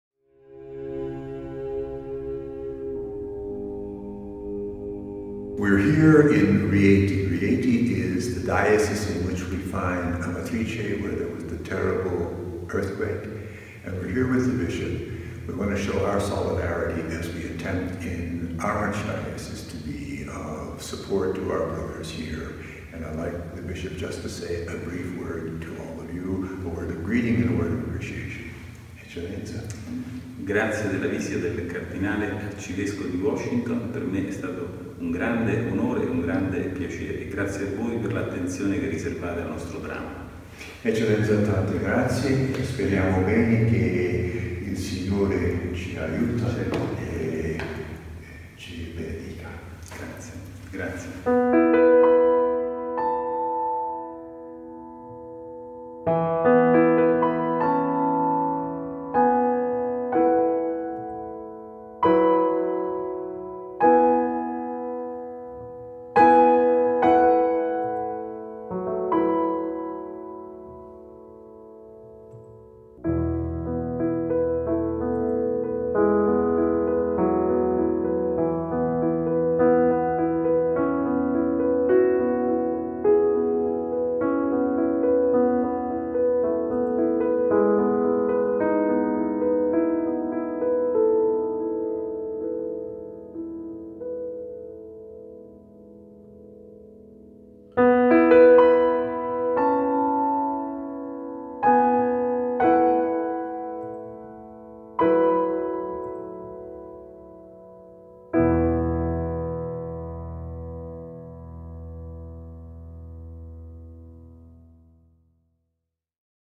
Cardinal Wuerl with Bishop of Rieti (Amatrice) | Cathedral and Crypt
Cardinal Wuerl meets with the Bishop of Rieti, Domenico Pompili and tours the Cathedral and Crypt after the earthquake in Amatrice in order to raise awareness about this tragedy.